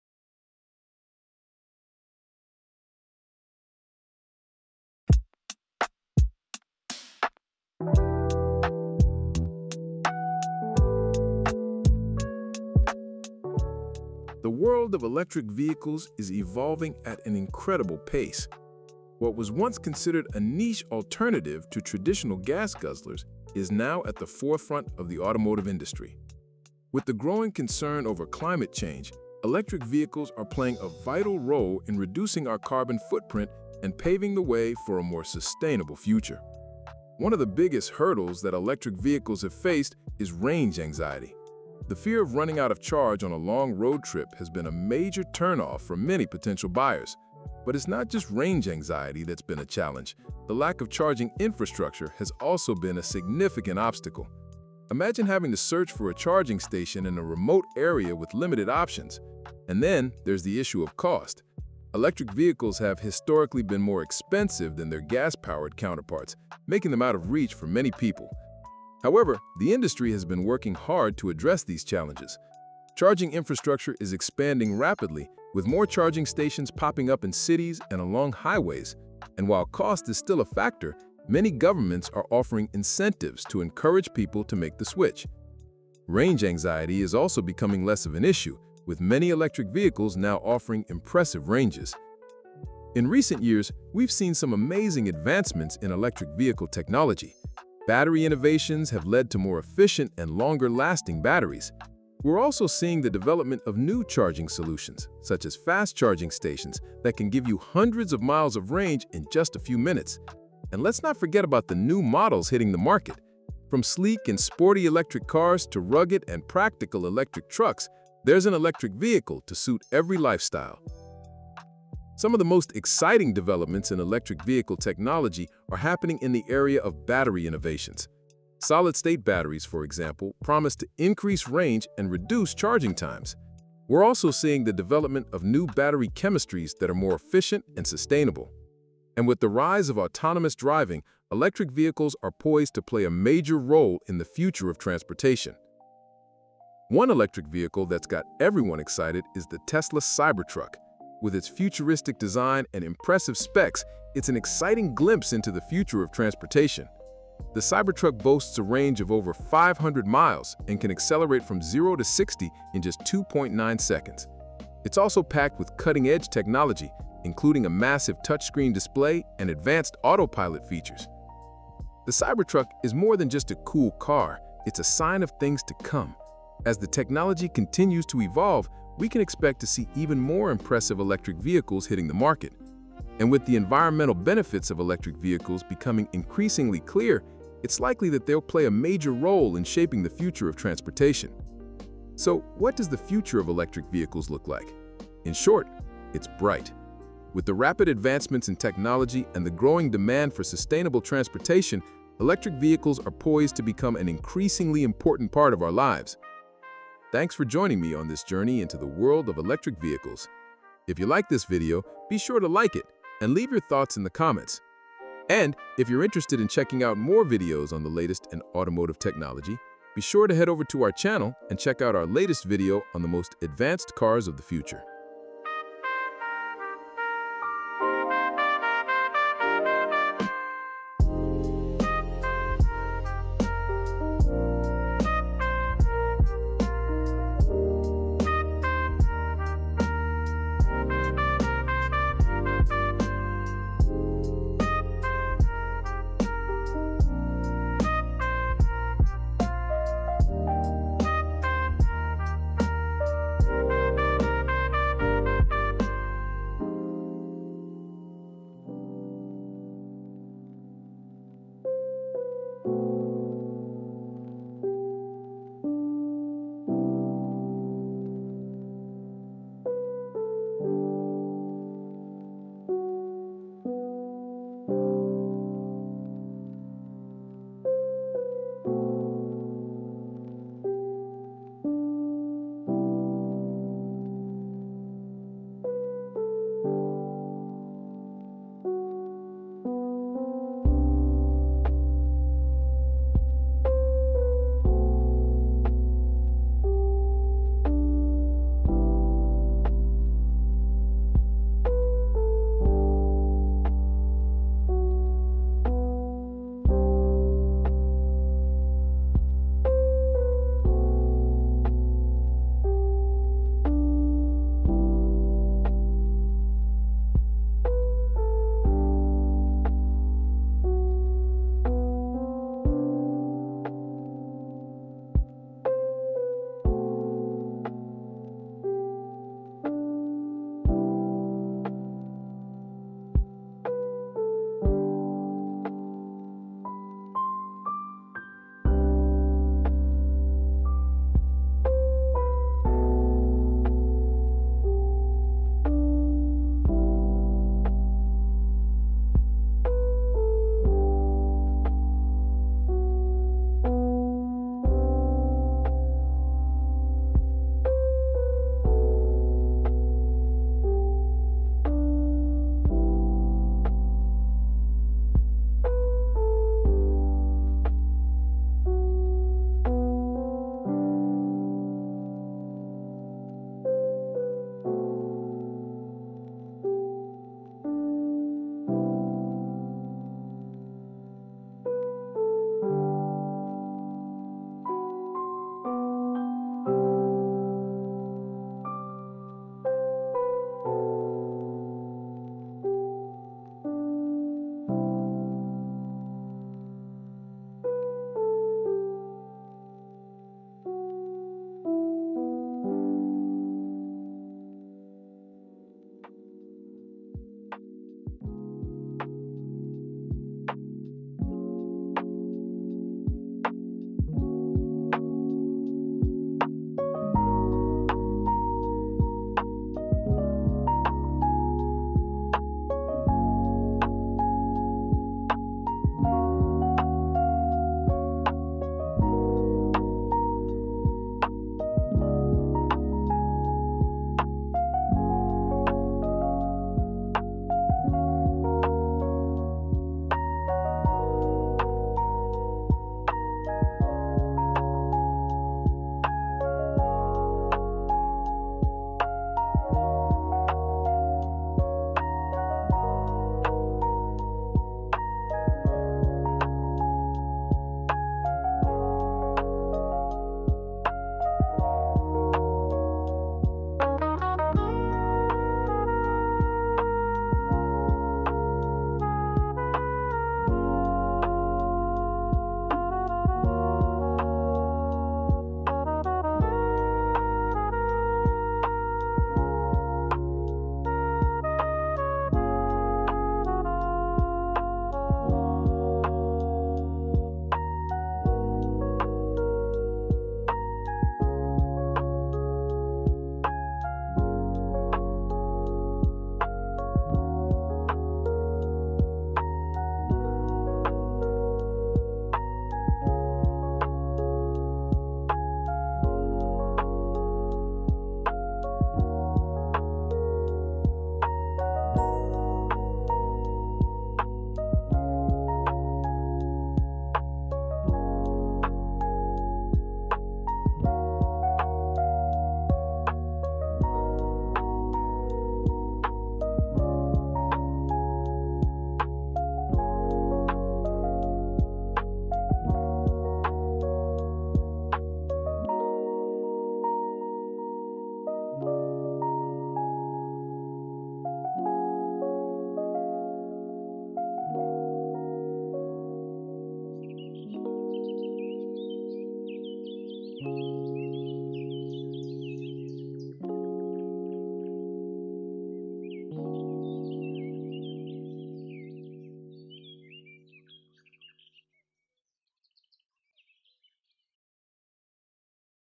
Electric Vehicles of the FUTURE Are Here Now! | Innovative Electric Vehicles
Get ready to rev up your excitement as we take a thrilling ride into the world of Electric Vehicles of the FUTURE! From sleek designs to mind-boggling tech, we're diving into the most revolutionary EVs that are changing the game RIGHT NOW! Join us as we explore the latest innovations, sustainable energy solutions, and what the future of transportation looks like. Buckle up and let's accelerate into the electric era!

Innovative Electric Vehicles

The world of electric vehicles is evolving at an incredible pace. What was once considered a niche alternative to traditional gas-guzzlers is now at the forefront of the automotive industry. With the growing concern over climate change, electric vehicles are playing a vital role in reducing our carbon footprint and paving the way for a more sustainable future.

One of the biggest hurdles that electric vehicles have faced is range anxiety. The fear of running out of charge on a long road trip has been a major turn-off for many potential buyers. But it's not just range anxiety that's been a challenge. The lack of charging infrastructure has also been a significant obstacle. Imagine having to search for a charging station in a remote area with limited options. And then there's the issue of cost. Electric vehicles have historically been more expensive than their gas-powered counterparts, making them out of reach for many people.

However, the industry has been working hard to address these challenges. Charging infrastructure is expanding rapidly, with more charging stations popping up in cities and along highways. And while cost is still a factor, many governments are offering incentives to encourage people to make the switch. Range anxiety is also becoming less of an issue, with many electric vehicles now offering impressive ranges.

In recent years, we've seen some amazing advancements in electric vehicle technology. Battery innovations have led to more efficient and longer-lasting batteries. We're also seeing the development of new charging solutions, such as fast-charging stations that can give you hundreds of miles of range in just a few minutes. And let's not forget about the new models hitting the market. From sleek and sporty electric cars to rugged and practical electric trucks, there's an electric vehicle to suit every lifestyle.

Some of the most exciting developments in electric vehicle technology are happening in the area of battery innovations. Solid-state batteries, for example, promise to increase range and reduce charging times. We're also seeing the development of new battery chemistries that are more efficient and sustainable. And with the rise of autonomous driving, electric vehicles are poised to play a major role in the future of transportation.

One electric vehicle that's got everyone excited is the Tesla Cybertruck. With its futuristic design and impressive specs, it's an exciting glimpse into the future of transportation. The Cybertruck boasts a range of over 500 miles and can accelerate from 0 to 60 in just 2.9 seconds. It's also packed with cutting-edge technology, including a massive touchscreen display and advanced Autopilot features.

The Cybertruck is more than just a cool car - it's a sign of things to come. As the technology continues to evolve, we can expect to see even more impressive electric vehicles hitting the market. And with the environmental benefits of electric vehicles becoming increasingly clear, it's likely that they'll play a major role in shaping the future of transportation.

So, what does the future of electric vehicles look like? In short, it's bright. With the rapid advancements in technology and the growing demand for sustainable transportation, electric vehicles are poised to become an increasingly important part of our lives.

Thanks for joining me on this journey into the world of electric vehicles. And if you're interested in checking out more videos on the latest in automotive technology, be sure to head over to our channel and check out our latest video on the most advanced cars of the future.